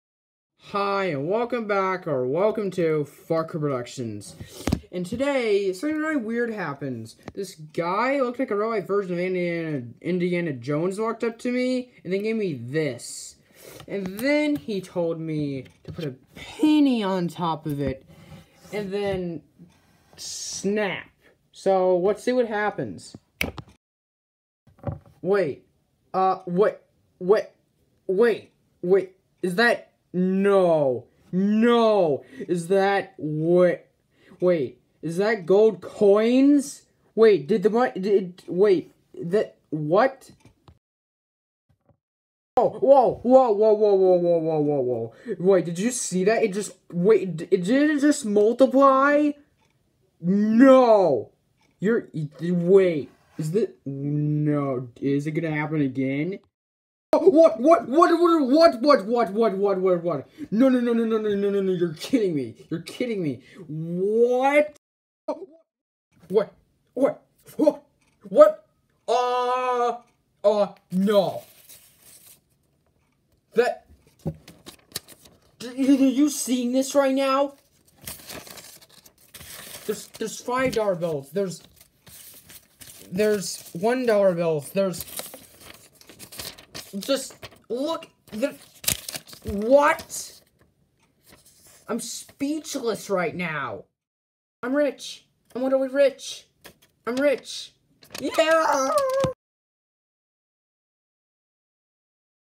The Money Is Multiplying 💰
Obviously I Don’t need to tell you This is fake money! And no I’m not trying to reference inflation.